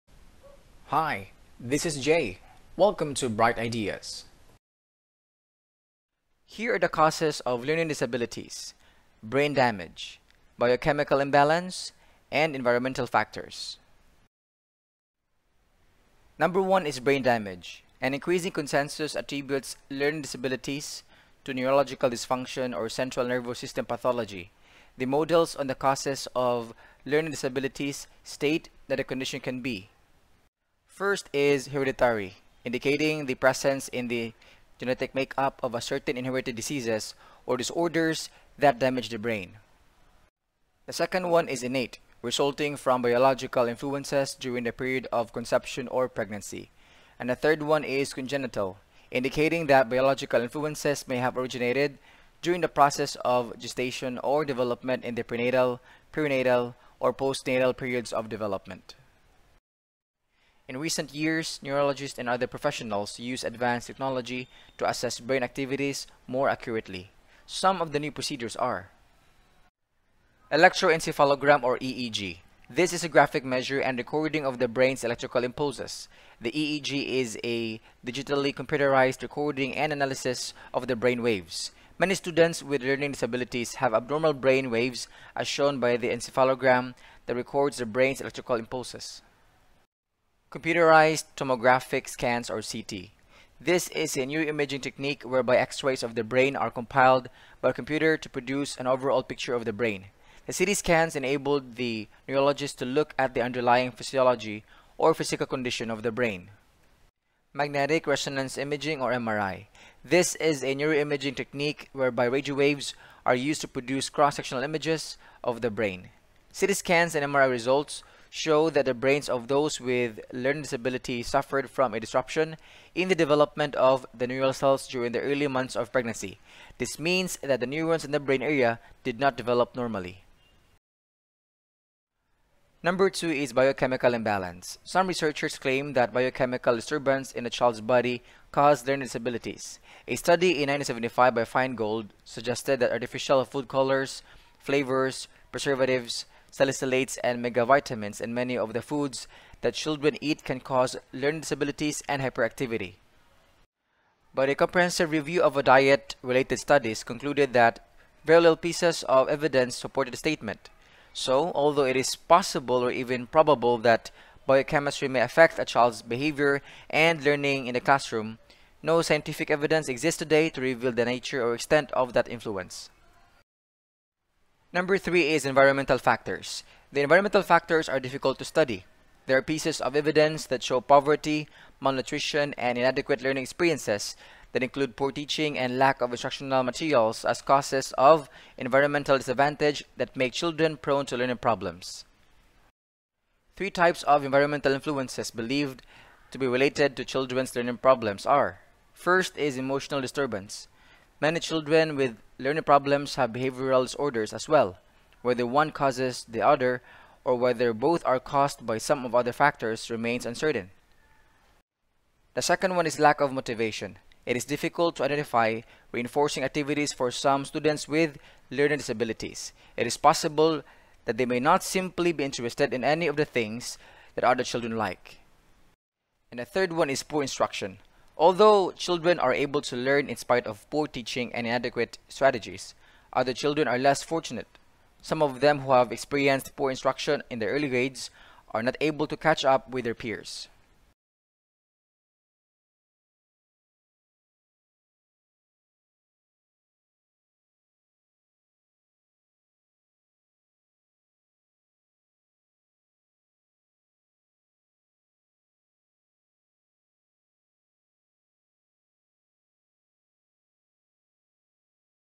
Genetic and Environmental factors in Learning Disabilities
This video explains the factors in Learning Disabilities such as:
1. Brain Damage
2. Biochemical Imbalance
3. Environmental Factors

Note: The content details are not mine. These are sourced from:
Introduction to Special Education  First Edition Book
Teresita G. Inciong
Yolanda S. Quijano
Yolanda T. Capulong
Julieta A. Gregorio
Adelaida C. Gines